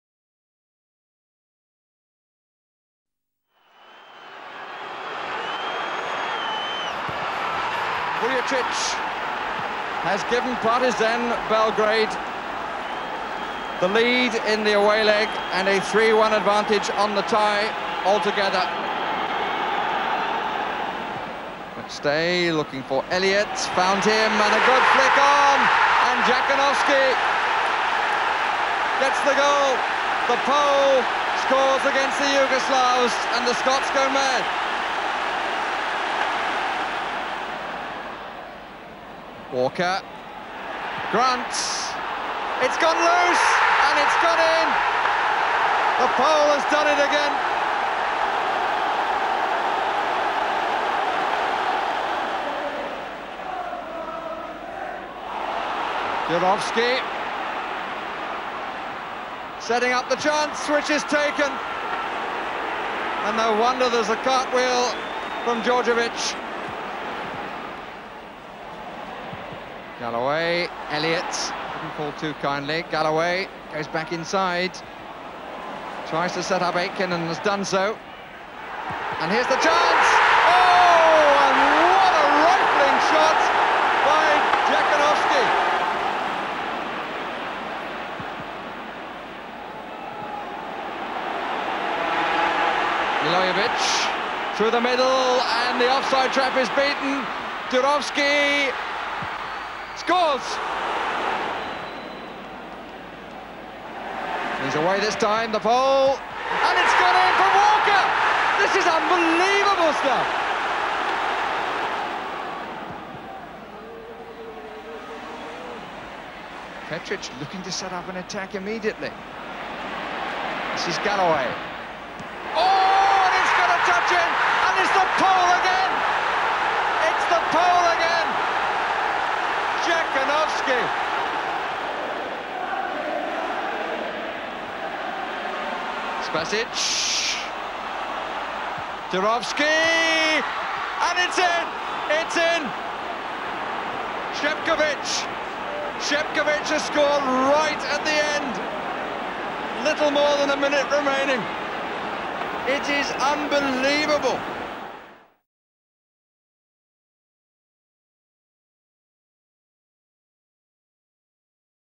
Classic Celtic Matches | Celtic 5-4 Partizan Belgrade | Dziekanowski scores FOUR! (27/09/1989)
It was on September 27, 1989, that Dariusz Dziekanowski scored four goals for Celtic in a 5-4 win over Partizan Belgrade, but the Hoops still went out of Europe on away goals following the epic 6-6 aggregate score.

Dziekanowski signed for Celtic earlier that season on July 14 from Legia Warsaw and it wasn’t long before he was christened ‘Jacki’ by supporters.

The Pole quickly became a cult hero with the Celtic fans and although his three years with the club may not have overflowing with games, goals or medals for the striker, he certainly caught the attention of the fans – and the media.